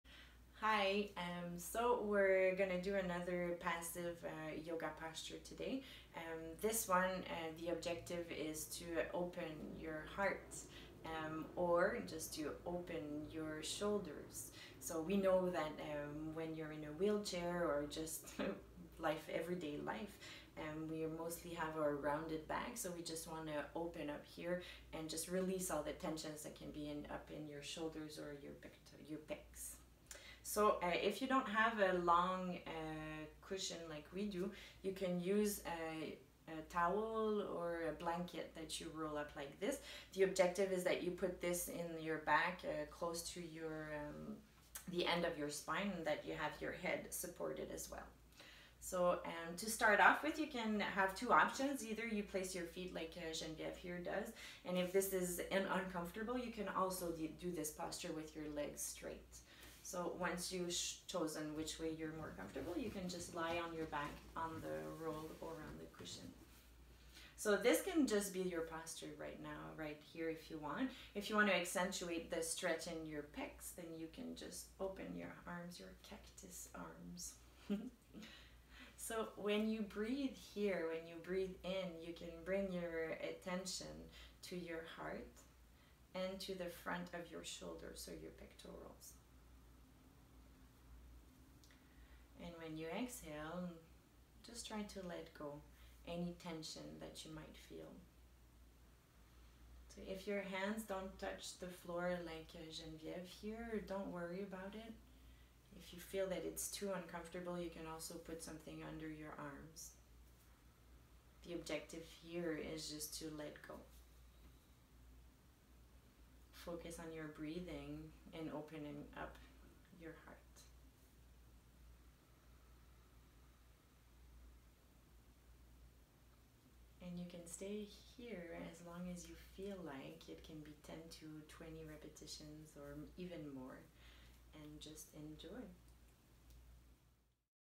Yoga chest opening
• Objectives: Opening of the heart, opening of the rib cage and shoulders
• Movement: Breathe with Ujjayi OR breathe normally. Concentrate on relaxing the pectoral muscles on expiration and open the heart to the sky. At each expiration, try to sink into the ground by the heaviness of your body.
• Tip: In case of discomfort, adopt the same opening posture, but in a semi-sitting position. Find the angle that allows you to relax tension and relax.